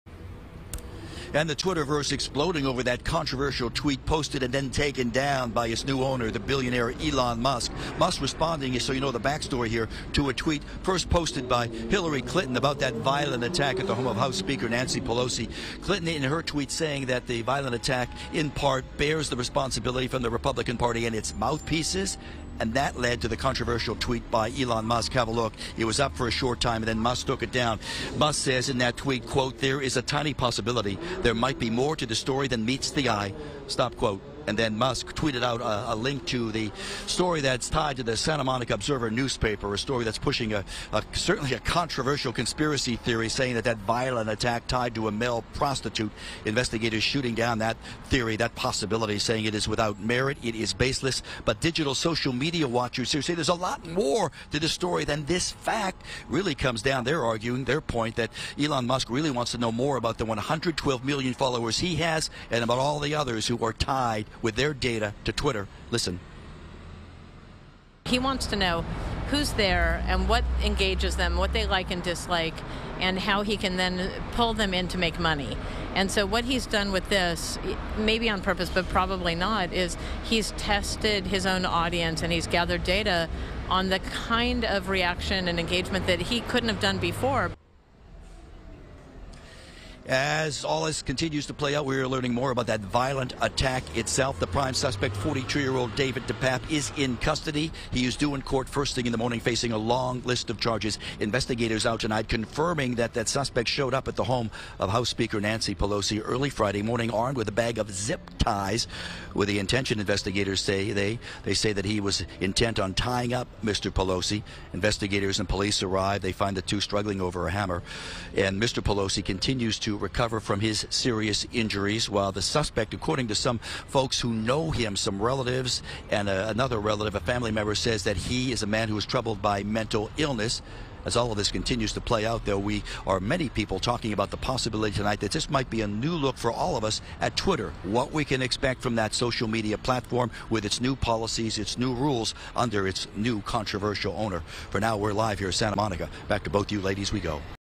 Musk tweets, deletes conspiracy theory about Paul Pelosi's attacker
Just days after buying Twitter, Elon Musk tweeted out — and then deleted — an article pushing an unfounded rumor about the attack against Speaker of the House Nancy Pelosi's husband, Paul Pelosi.